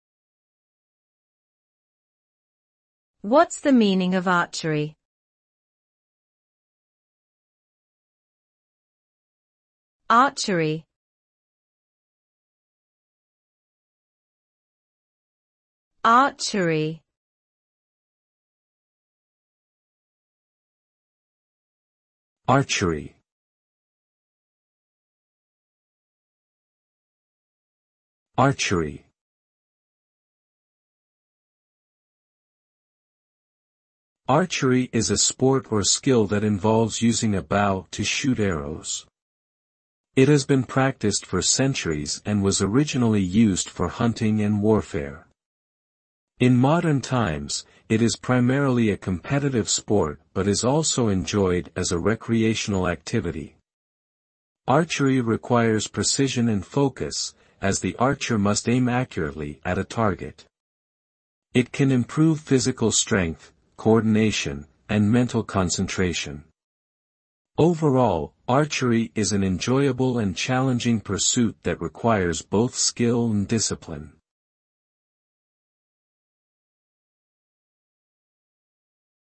What's the meaning of "archery", How to pronounce it?
This video tells you what "archery" means, how to pronounce it, what is "archery" definition, provides a pronunciation guide and an accurate definition.